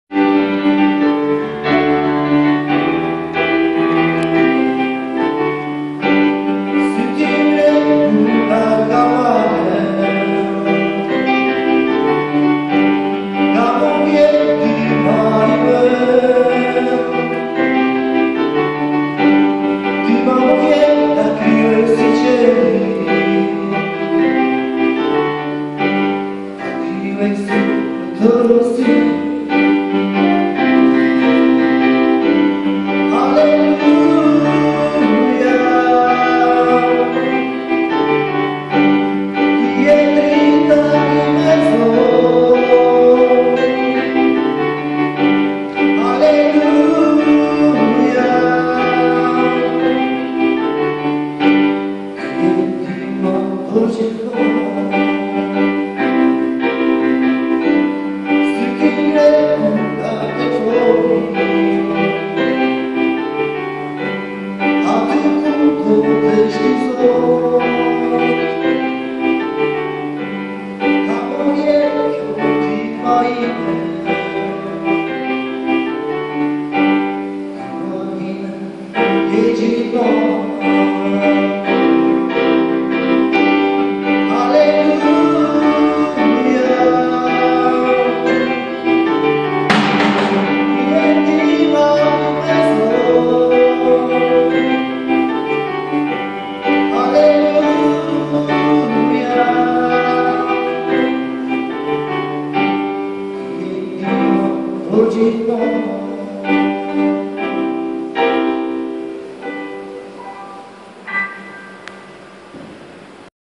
AKON15 - Rudi Shkullaku
On the Allianz-Conference in Bad Blankenburg Rudi Shkullaku performed his self-written song in albanian language.
Sorry for the bad videoquality...